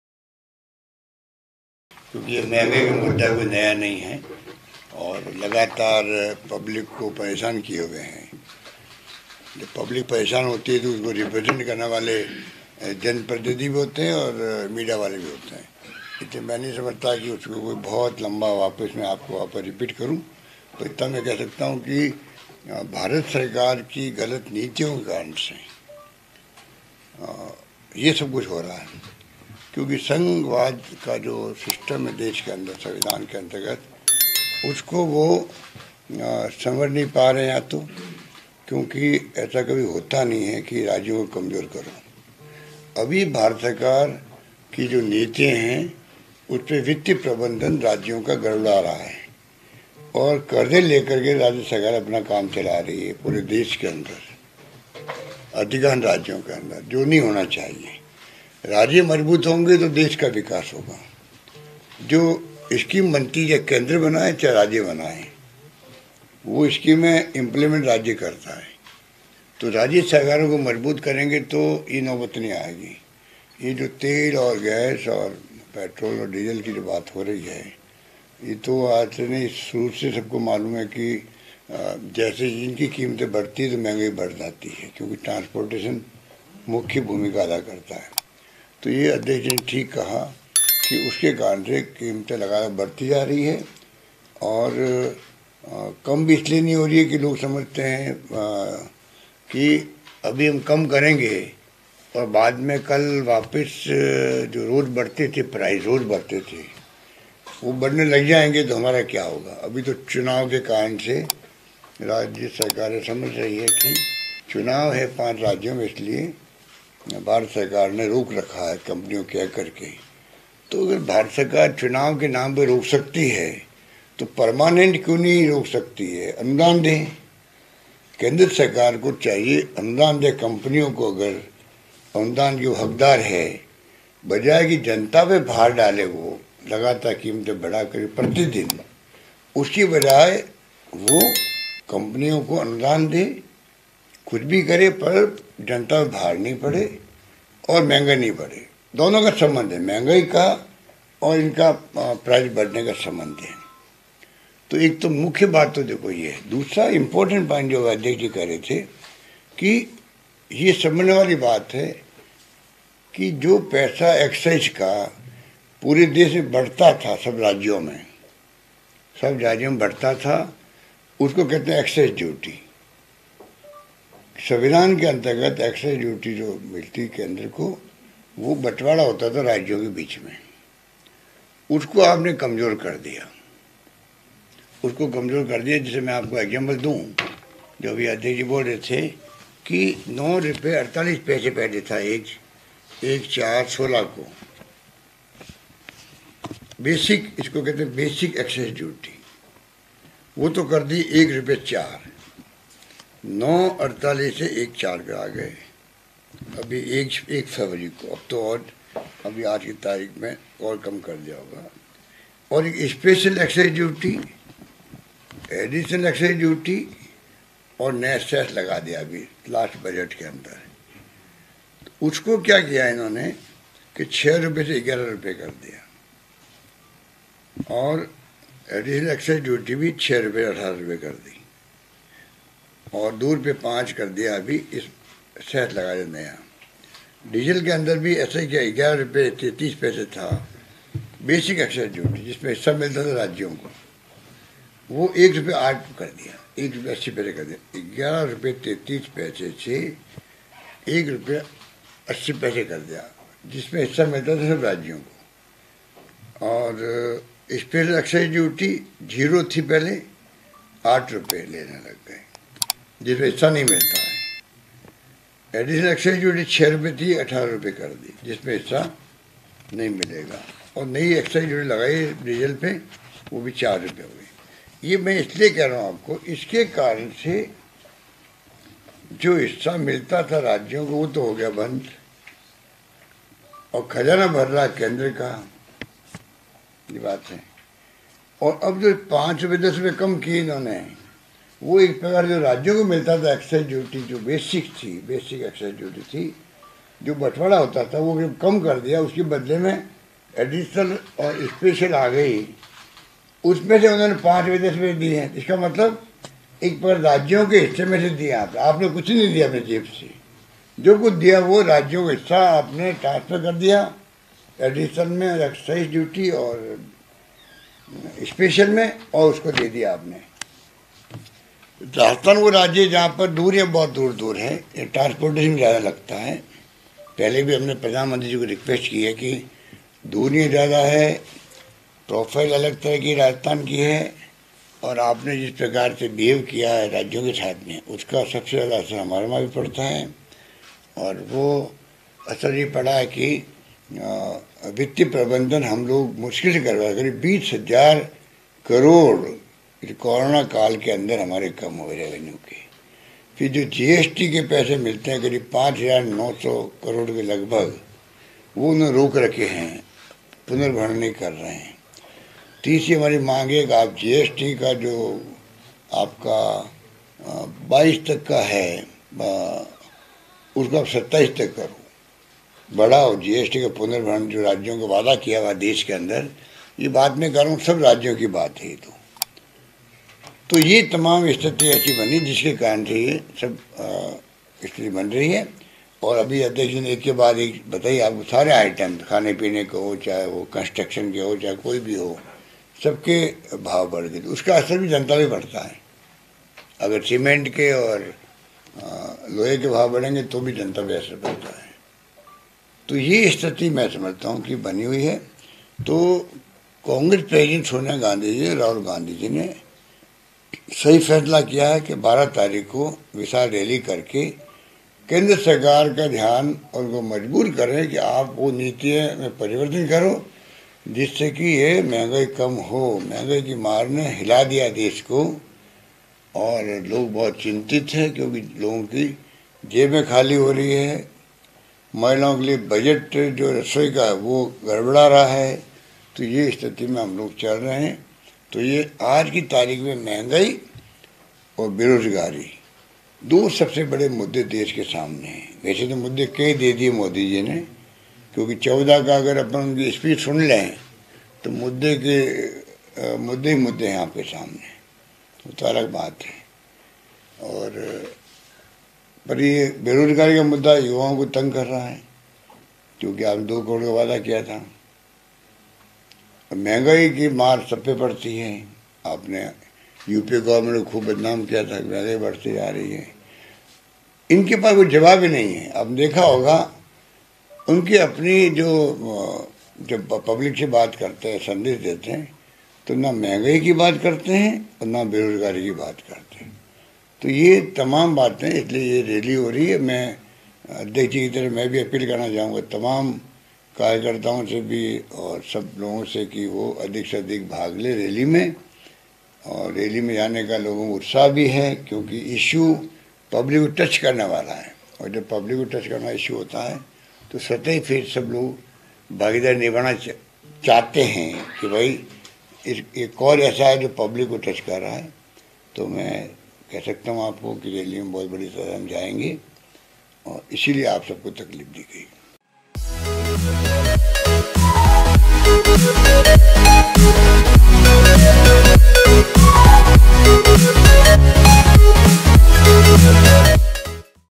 अशोक गहलोत ने कहा राज्यों को कमजोर कर रही है केन्द्र की मोदी सरकार | Ashok Gehlot on Narendra Modi
अशोक गहलोत ने कहा राज्यों को कमजोर कर रही है केन्द्र की मोदी सरकार